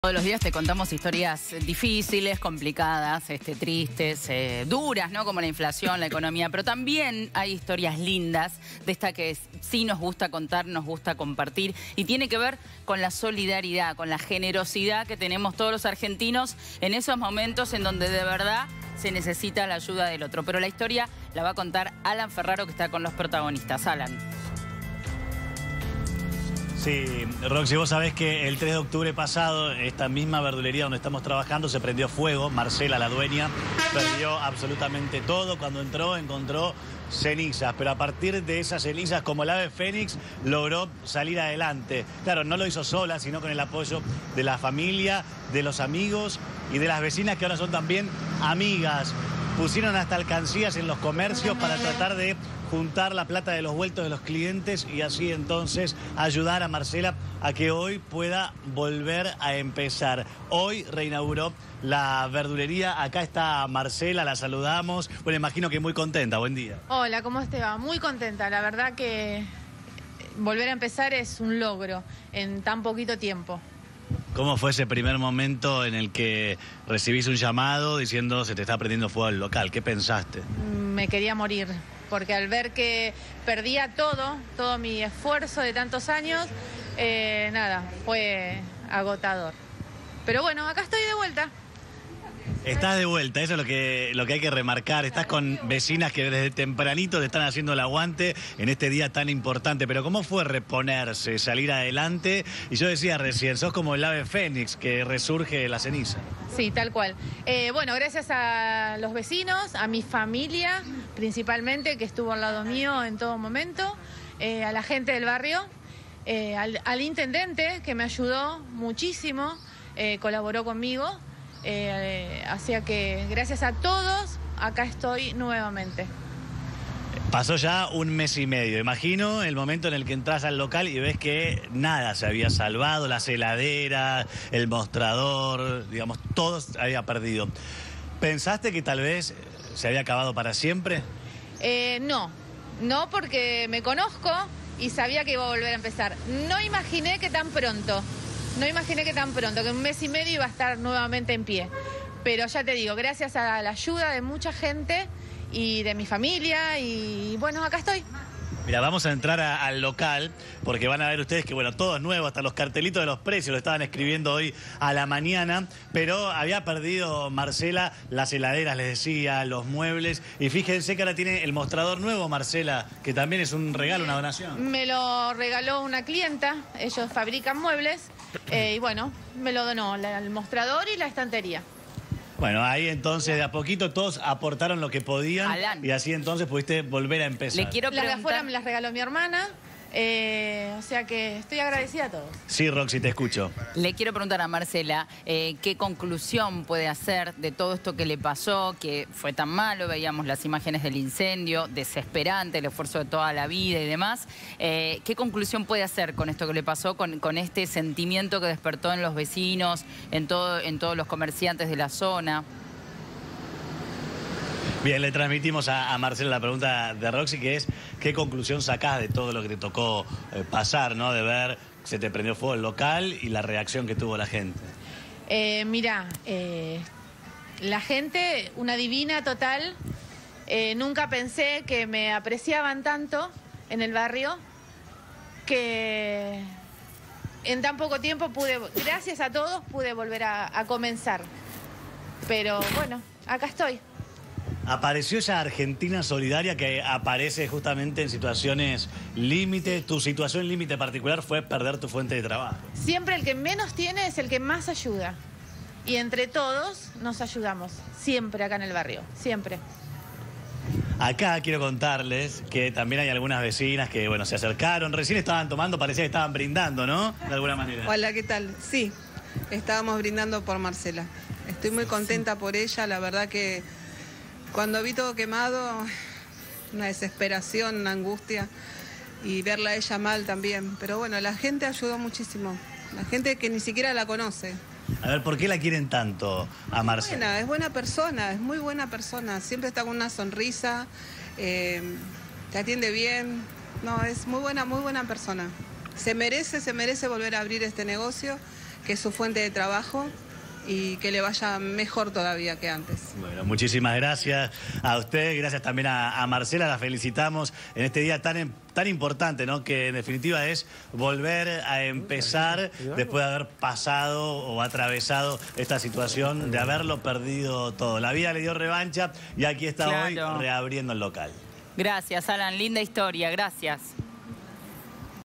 TRES DE FEBRERO I Por un incendio perdieron su verdulería y gracias a su vecinos volvió a abrir
Hace un mes, se prendió fuego su fuente de trabajo y gracias a sus vecinos que los ayudaron a organizar una colecta, pudieron reabrir nuevamente.

Marcela - dueña de la verduleria -

"No imaginé que en un mes y medio iba a estar otra vez de pie"

"Nunca pensé que me apreciaban tanto"